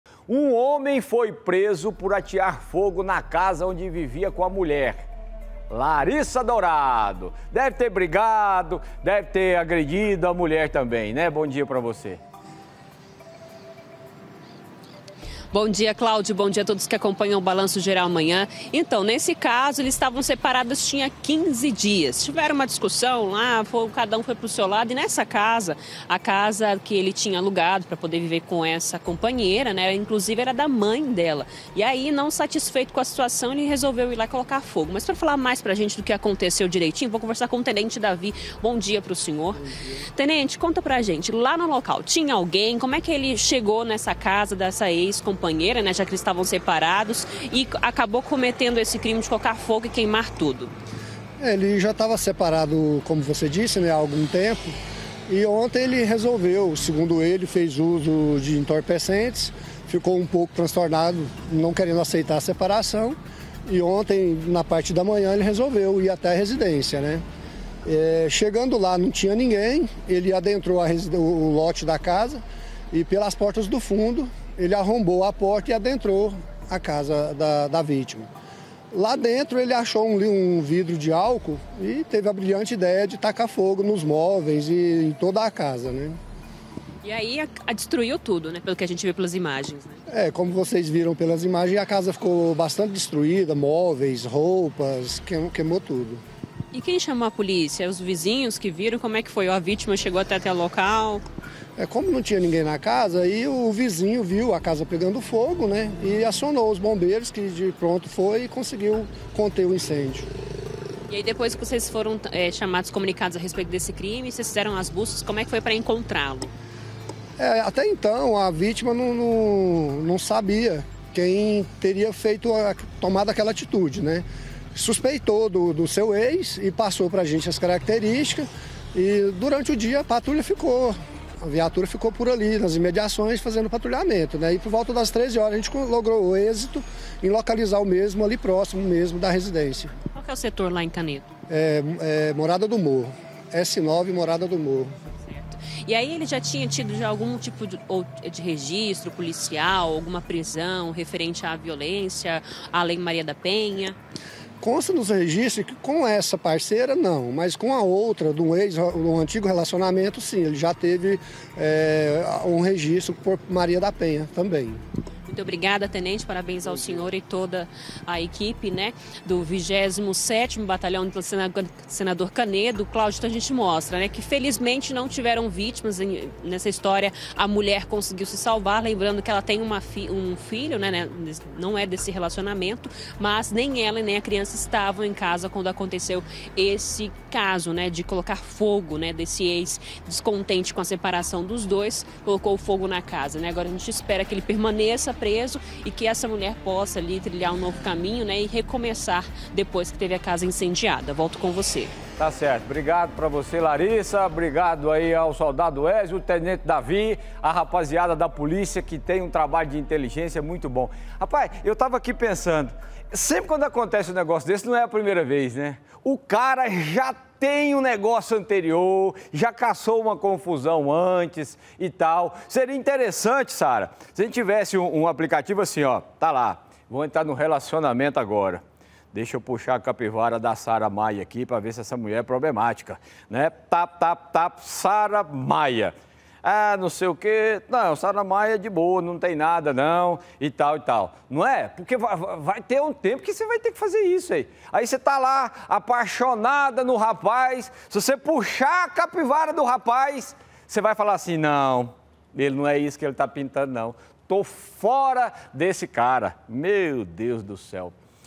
INCENDIÁRIO: HOMEM BRIGA COM MULHER E PÕE FOGO EM CASA
UM HOMEM FOI PRESO POR ATEAR FOGO NA CASA ONDE VIVIA COM A MULHER.